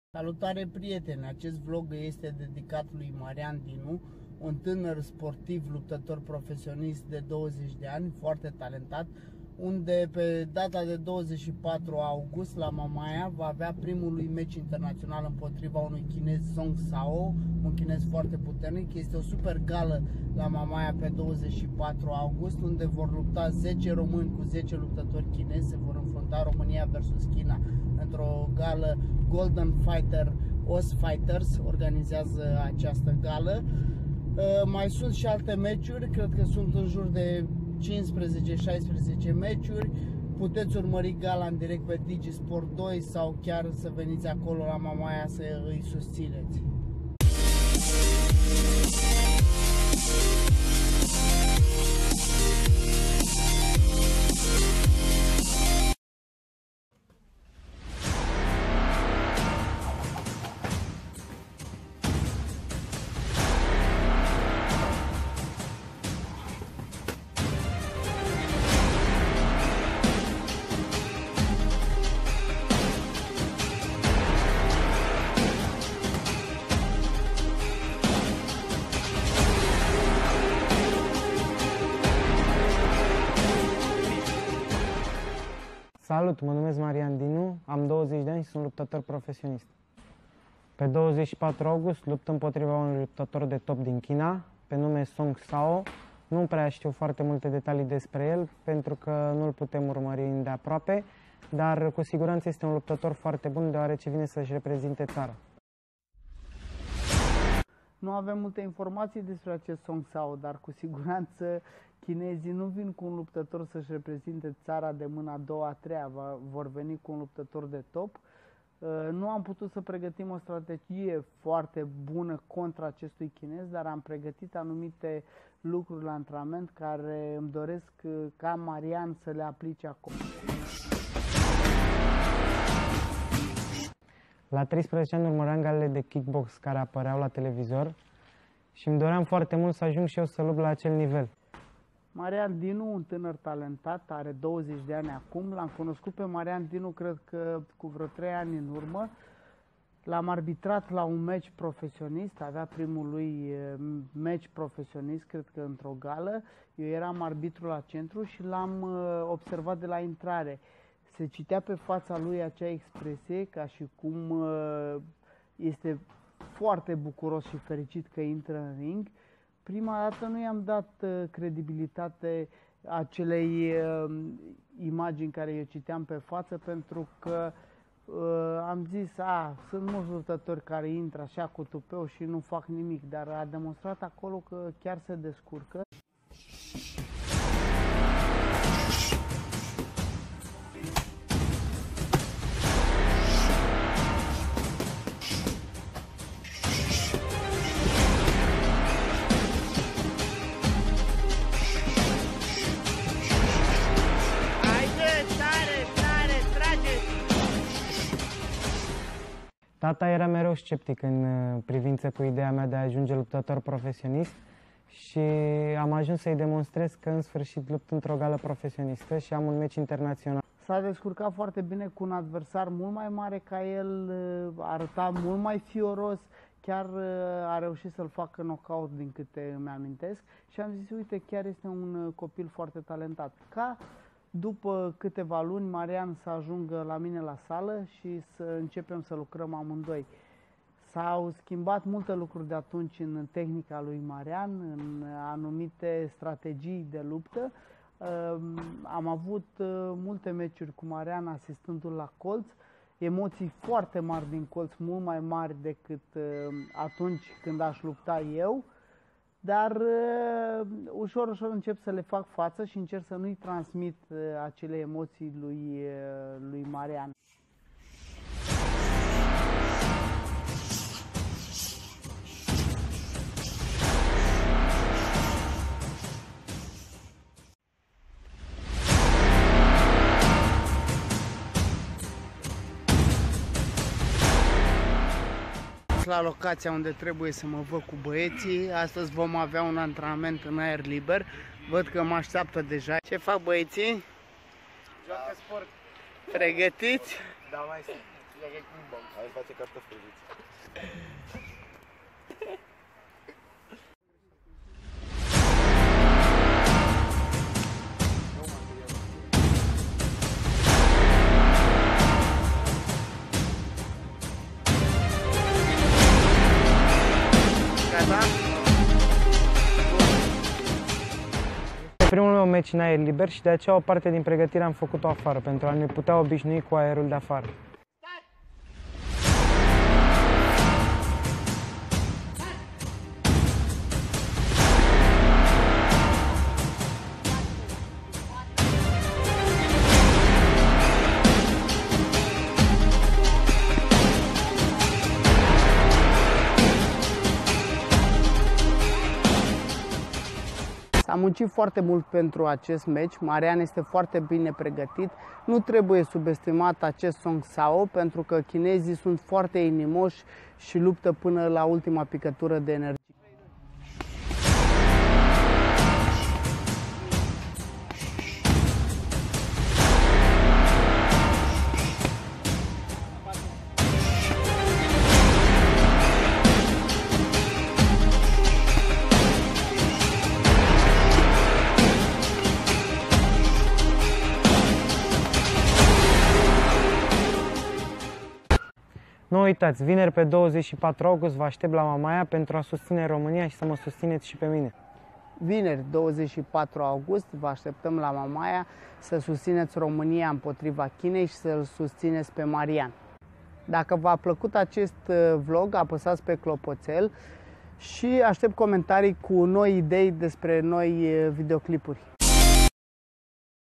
Episod dedicat lui Marian Dinu
Pregătire pentru gala OSS Fighters GOLDEN fighter Romania Vs China.